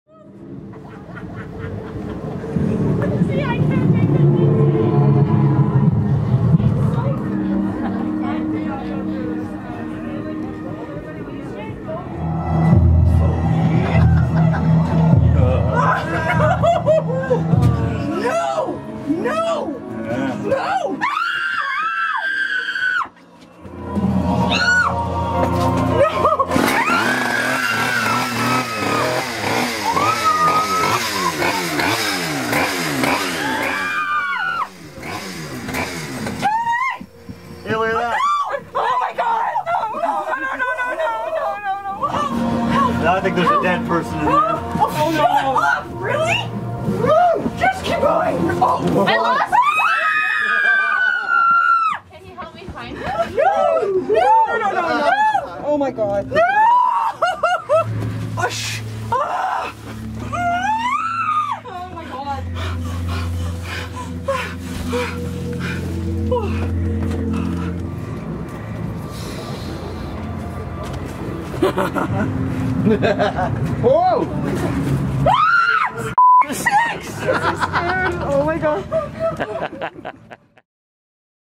Brinkworth Dungeon Haunted House
Brinkworth Dungeon is a Halloween haunted house in Metro Vancouver. It's located at 22260 48th Avenue in Langley.

From Sundays to Thursdays it only has one or two live characters in costume. Those days are when they recommend more timid folk visit. Fridays and Saturdays are when they have up to six or so live actors. As you'll see in this video, the actors make the place especially terrifying.